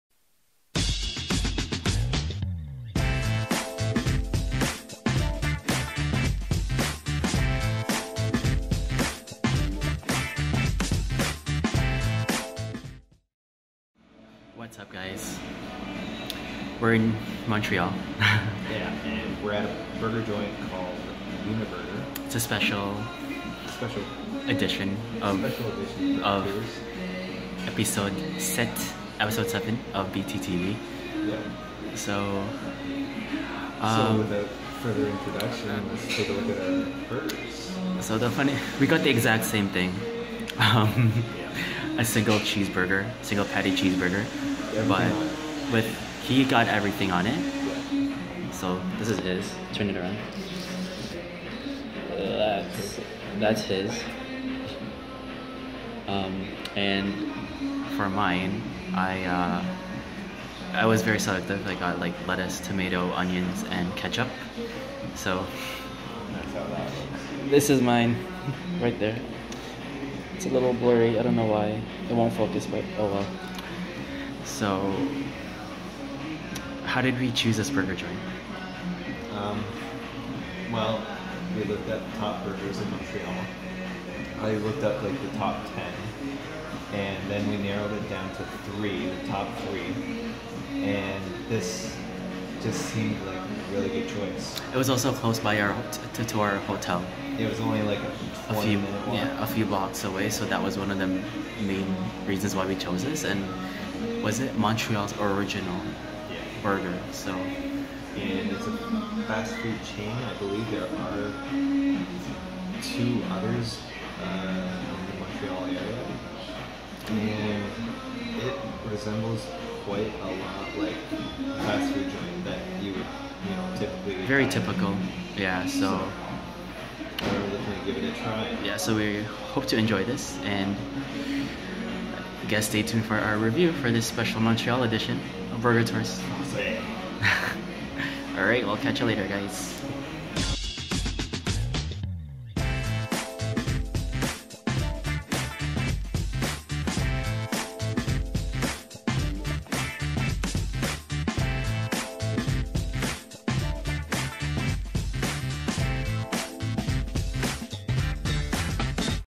BurgerTourTV Episode 7 - Uniburger
In this special episode of BurgerTourTV, Andrew and Mark travel to Montreal to try from an original burger chain, Uniburger.